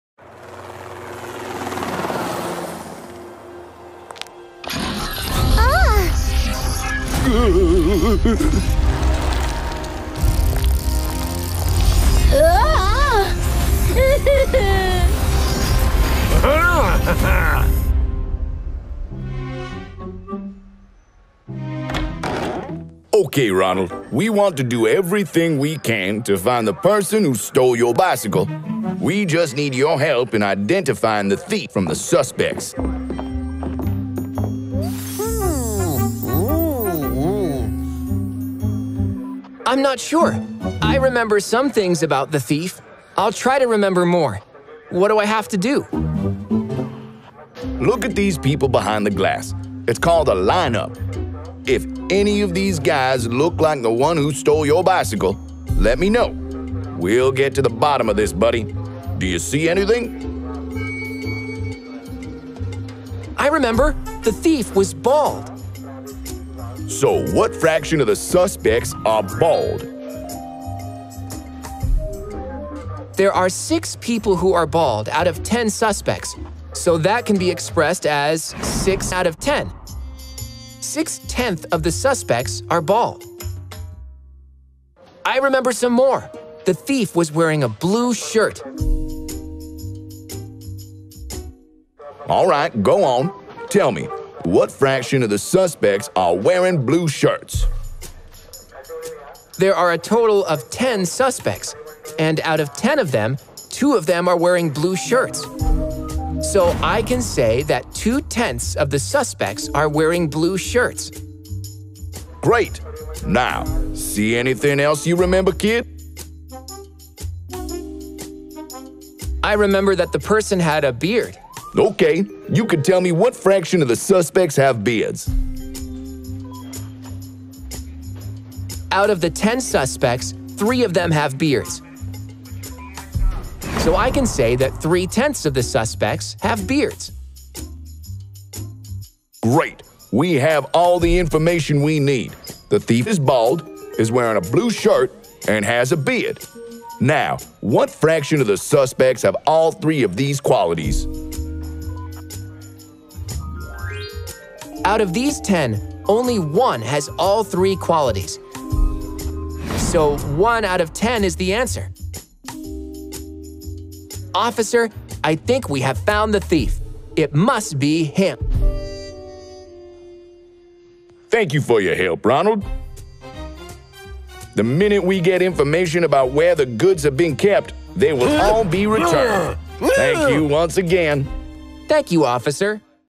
Find The Thief With Ronald | Fractions | BEL - K3
"Your BYJU'S learning companion Ronald’s bicycle was recently stolen, and he is at the police station to identify the thief. But, there's a problem. He only remembers a few details about the thief and needs your help in identifying him from the lineup of suspects. Use fractions and help our friend Ronald find the bicycle thief!"